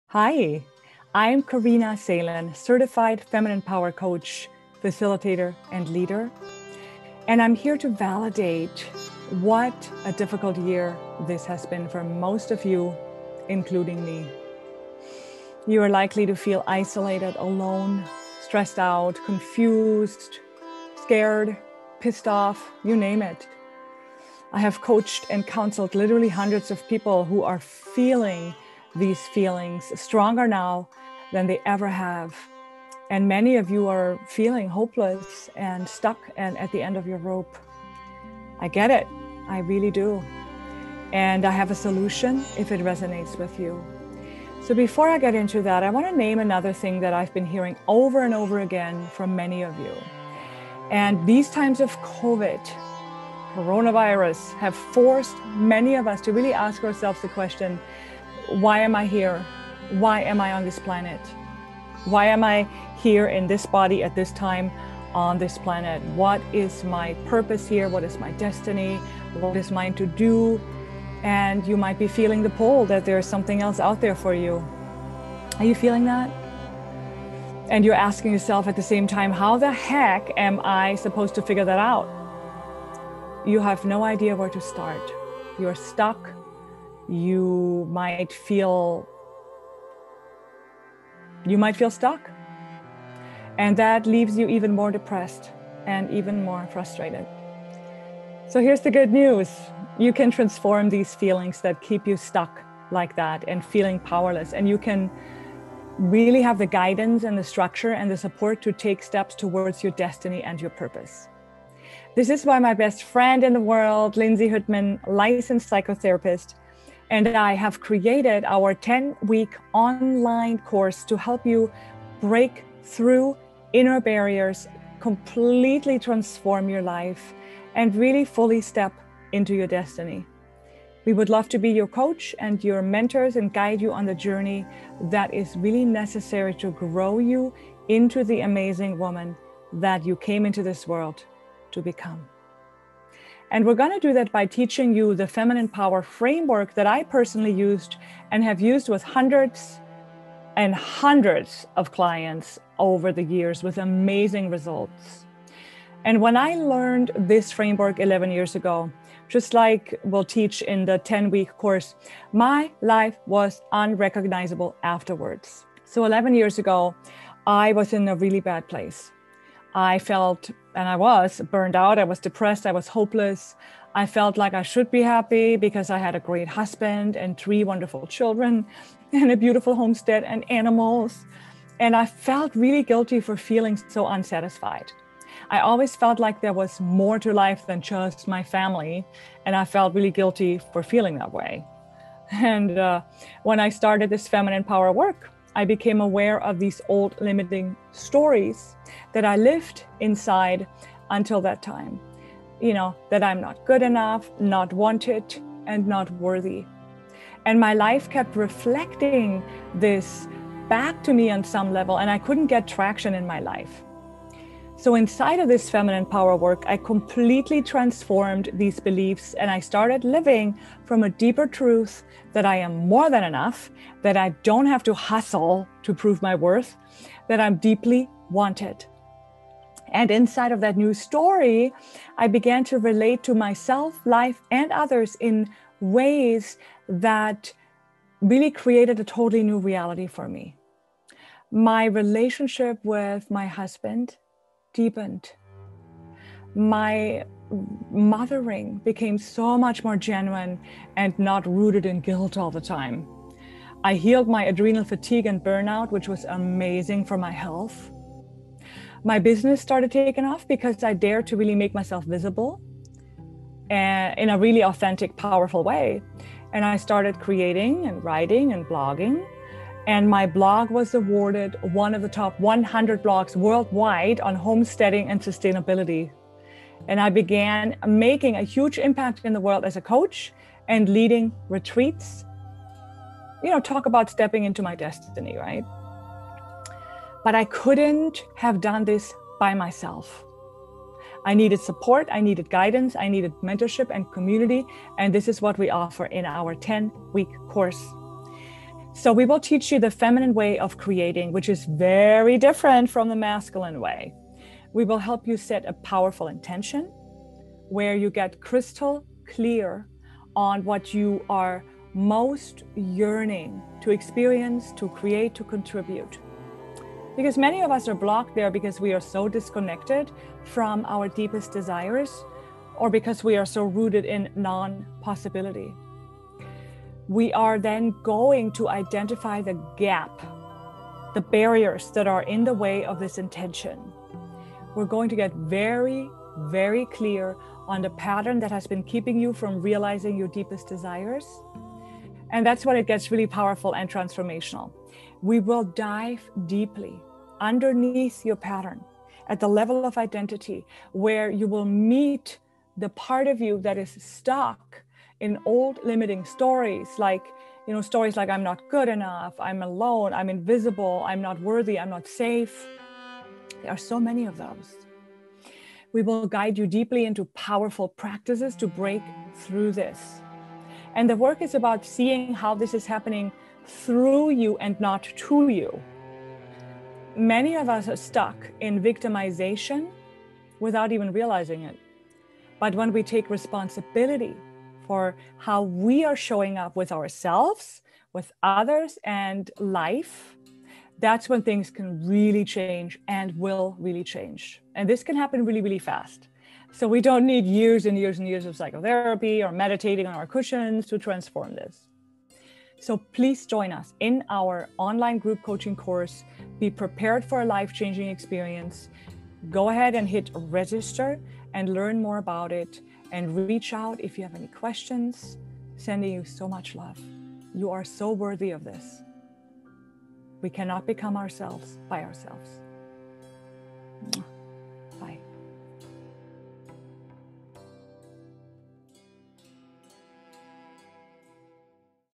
Ten Weeks Total Life Transformation: Intention, Clarity, Manifesting Online Course
Are you feeling alone, trapped and stuck in your life? Are you yearning to realize your potentials and your destiny?
I want to invite you to join our ten week online group coaching program. In it, we will teach you tools and principles to radically transform your life and to realize your intentions.

This work is incredibly powerful and life changing.  Inside of it, I have evolved my own painful patterns of believing I am not good enough, not worthy, and not wanted and created several very powerful transformations that are now reflected in my amazing life.

We can't wait to meet you and help you get clear on what exactly you want to create in your life, craft a powerful intention for our ten weeks together, get to know the underlying belief patterns that keep creating the same old pattern that's been holding you back, and then teach you how to transform your relationship with yourself, others and the greater field of life.